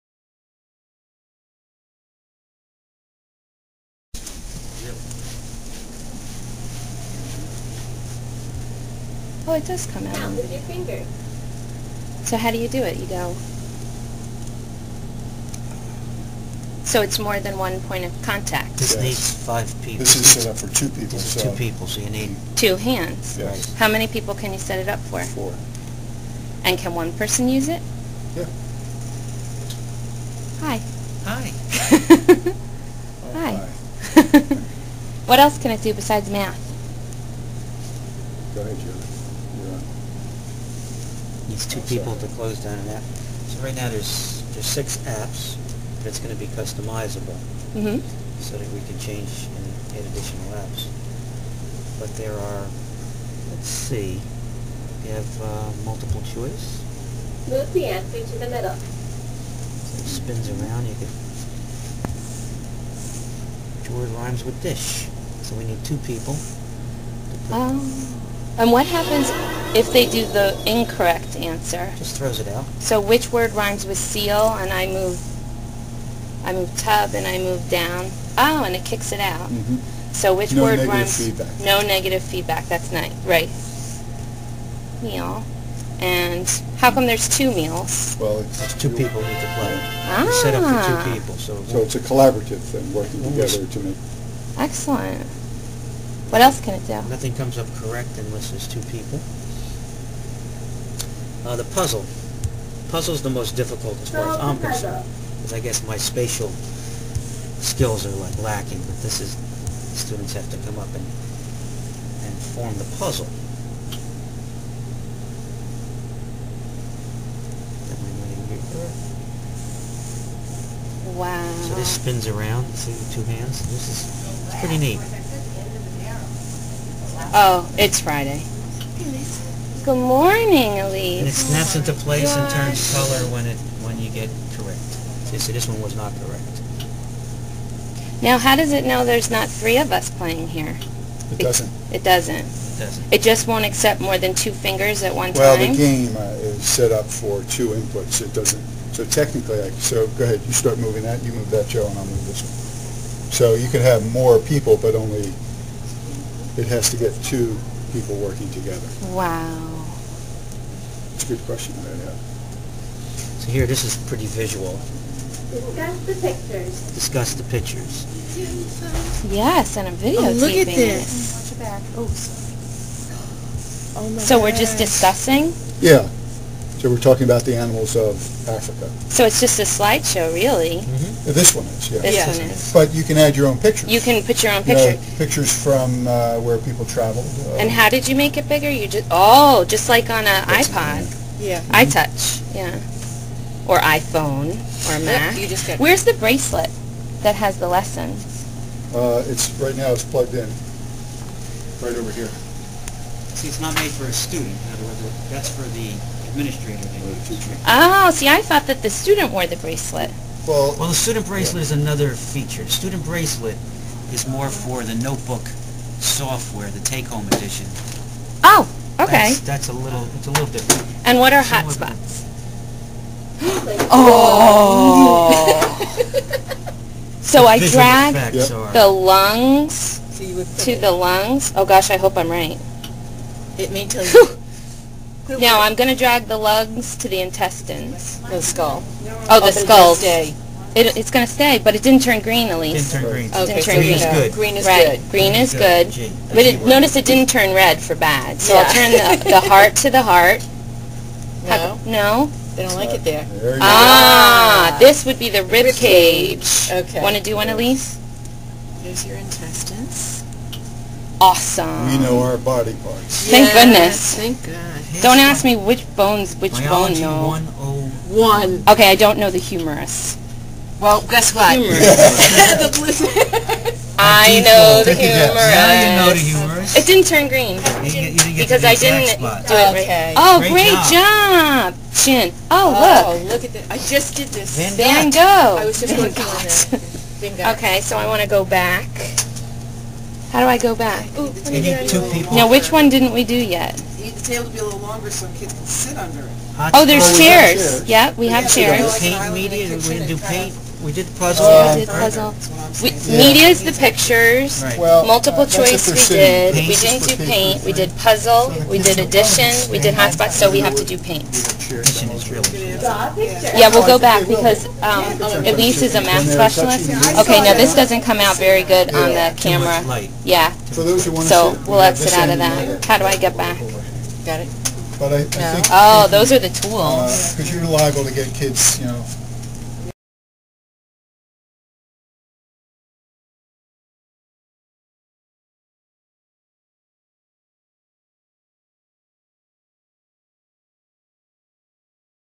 SMART Table at CMSCE, Rutgers University
This is a narrated tour of the SMART Table at a SMART Board Showcase at the CMSCE, Rutgers University. Wayne Copeland and Joe Ferraro from Keyboard Consultants provided Lisa Thumann, Sr. Specialist of Technology Education at the CMSCE, with the tour.